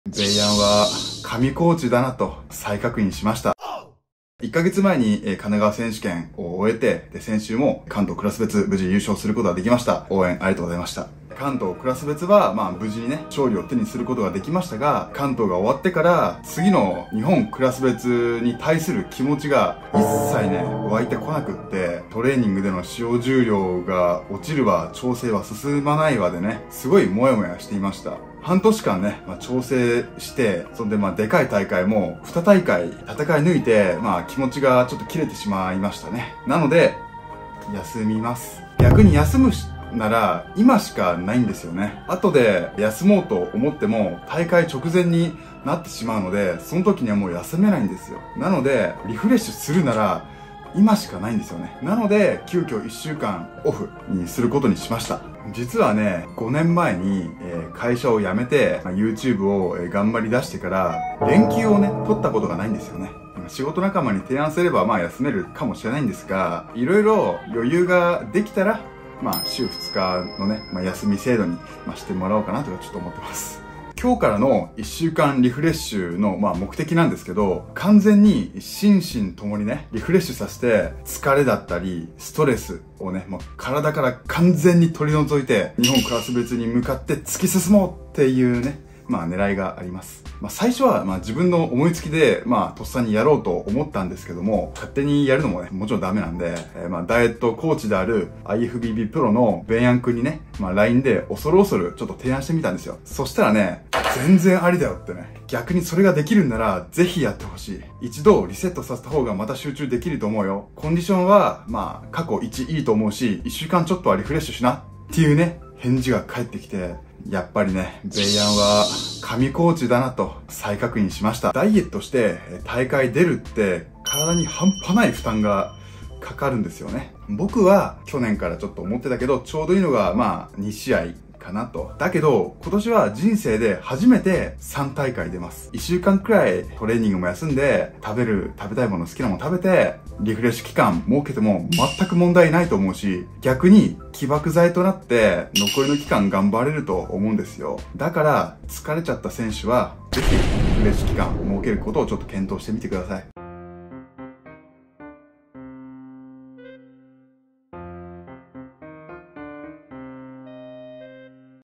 【疲れた人へ】 リフレッシュの大事さを語る芳賀セブン 【切り抜き】筋トレ
【概要】
毎日20時投稿しています。

芳賀セブン様の面白いところを主に切り抜きをしています。

コメント欄も動画に関する感想をお聞かせ頂けると嬉しいですm(__)m

※ご本人様に許可済みです。